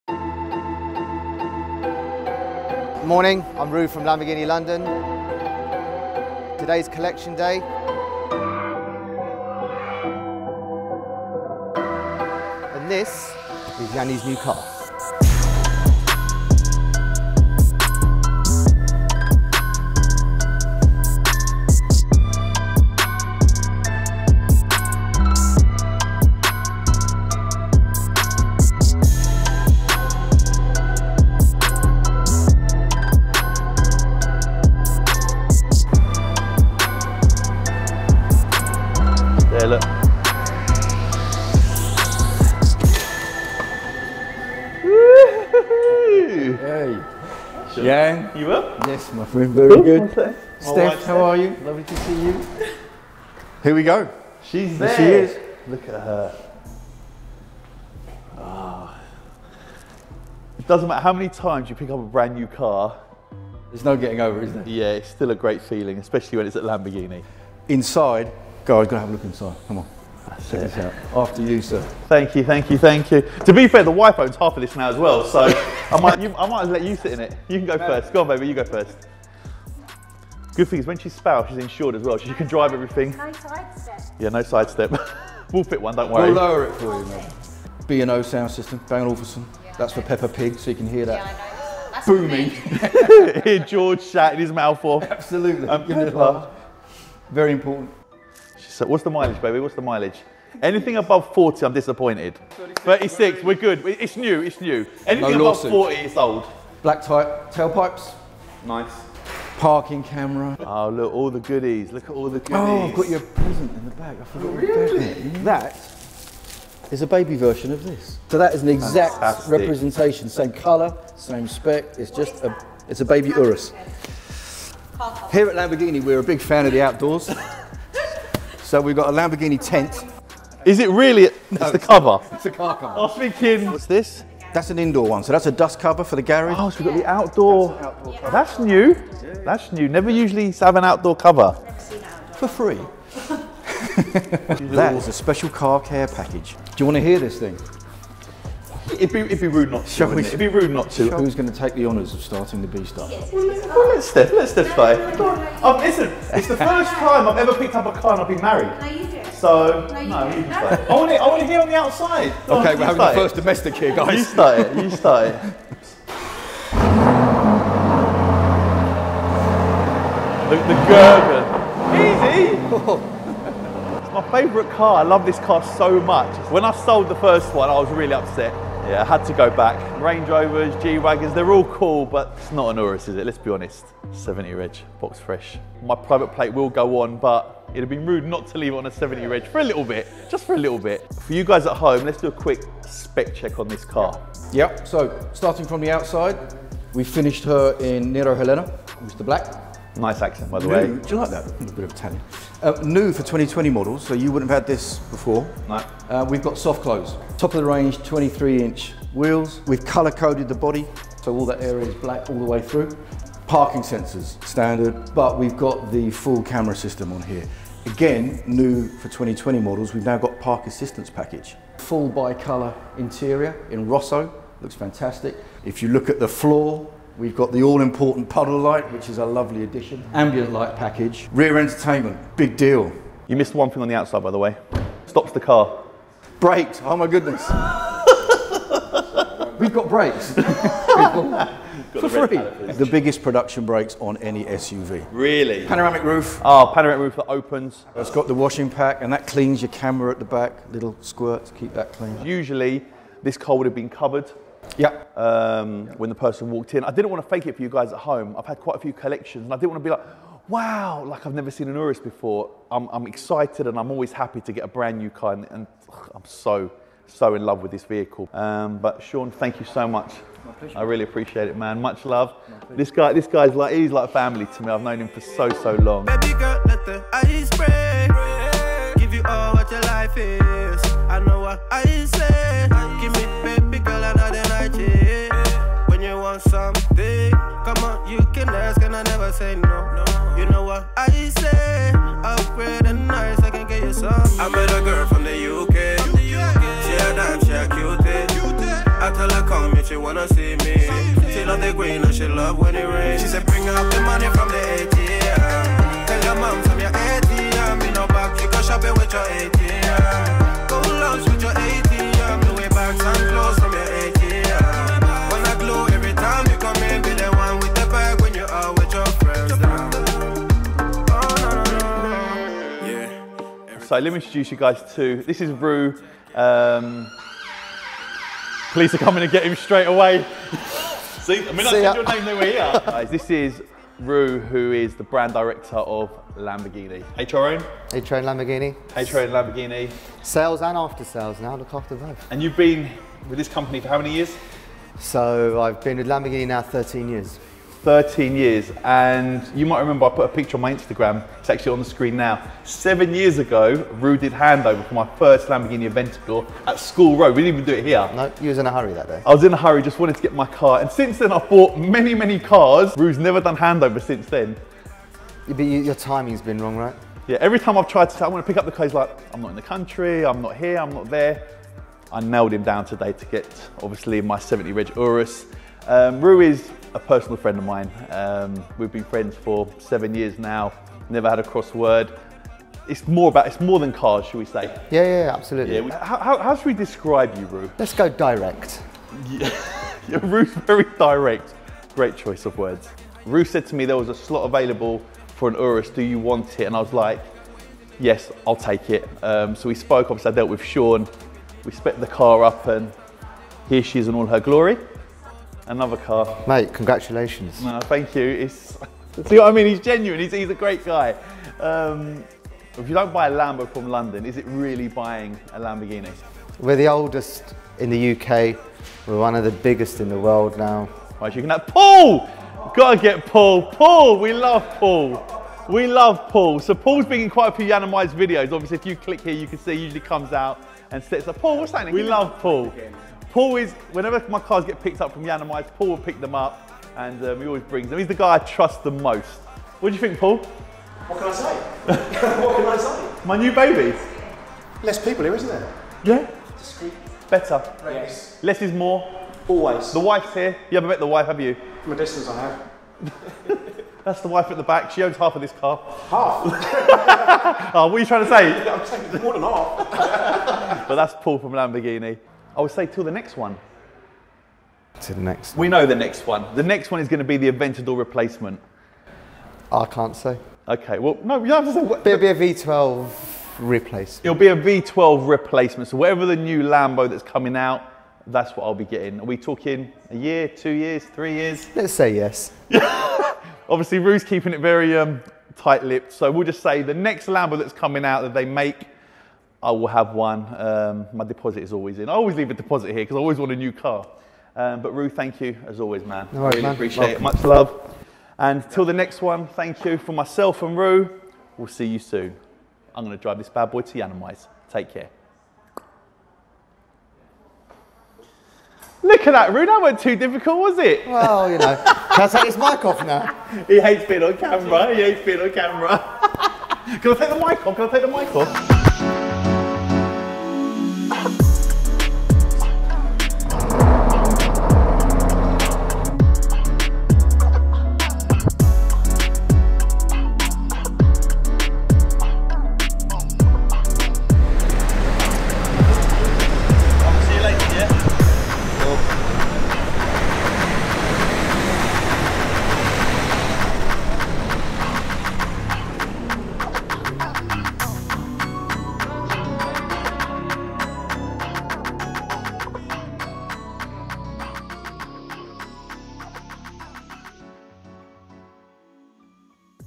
Collect My New Urus... Again
Having already done an Urus collection and reveal video 2 years ago, it didn't feel right to do the typical 'car reveal video'. Instead I collected my new 70 reg Lamborghini Urus from HR Owen with minimum fuss. :)

:: HR OWEN ::
IG » @Hrowenlamborghini